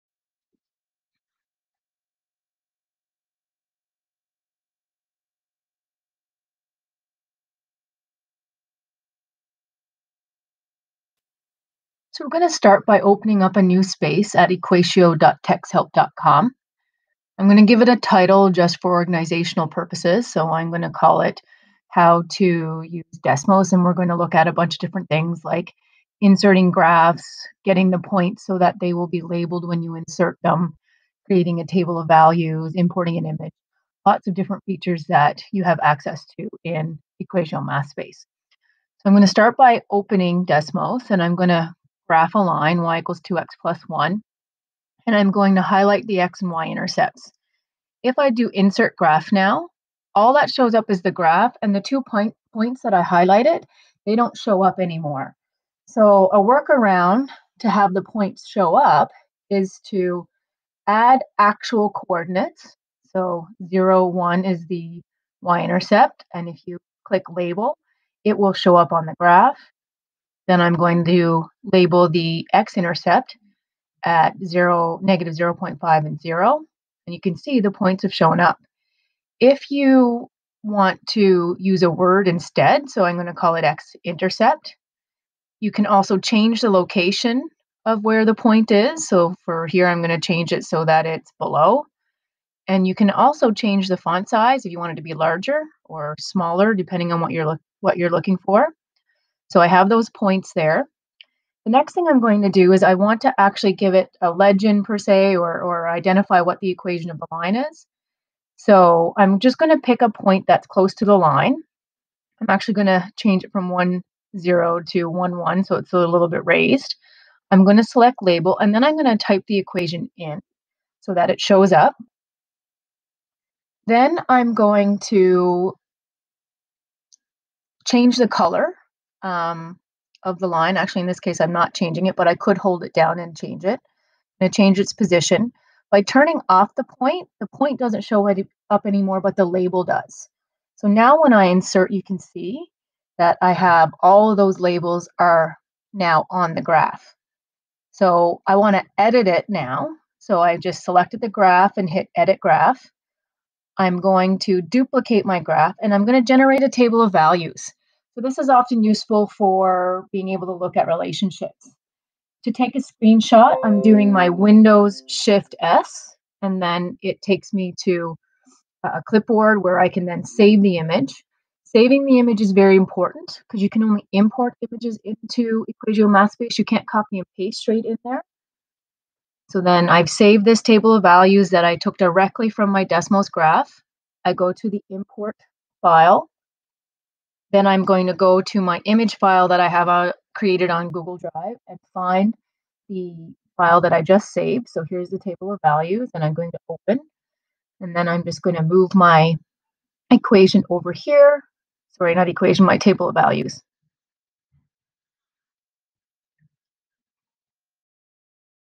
ocsbHowTo Use EquatIO Mathspace with Desmos
Leverage Demos graphing functions in Math Spaces.

Add points on a line with coordinates and with labels.
Add a Legend for the equation of the line. (0.57)
WINDOWS + SHIFT + S = screenshot for table of values (2.40)